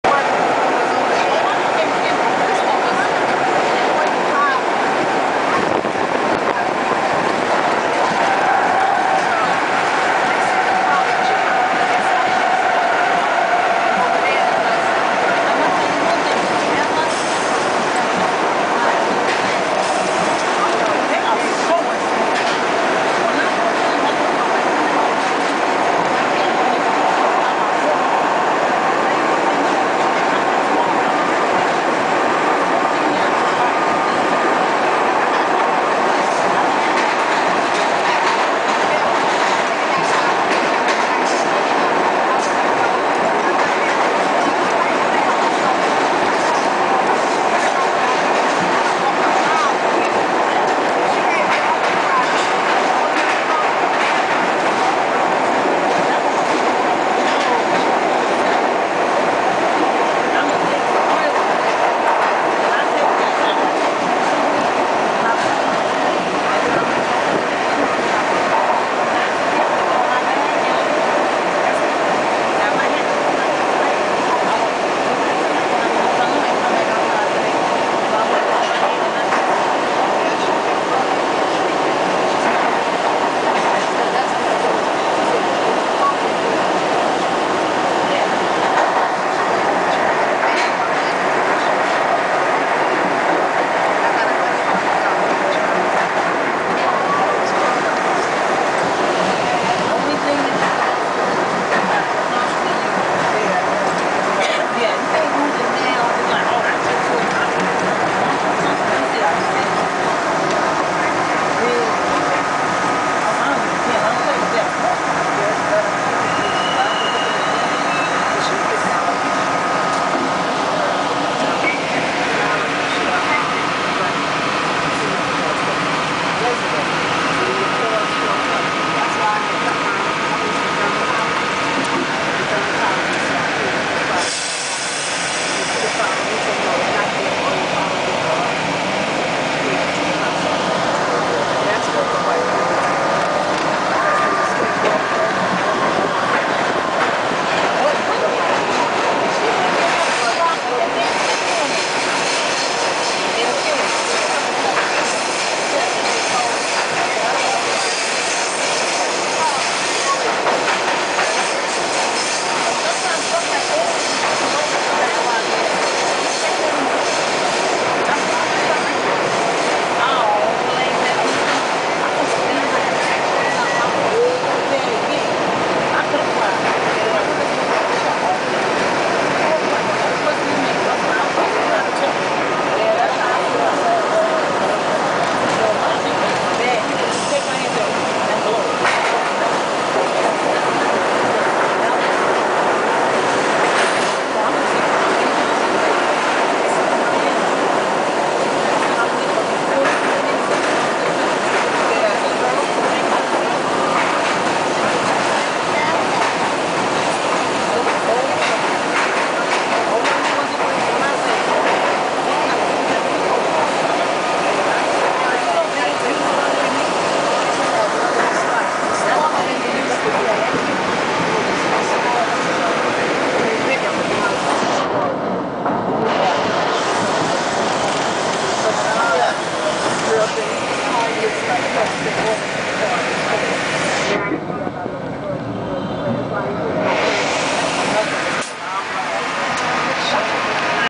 Riding the NY Subway
Never been to New York? Always wondered what it was like to ride in the NYC subway?  Well here's your chance!  Here's what it's like to ride in the first car.  Now you're a real straphanger!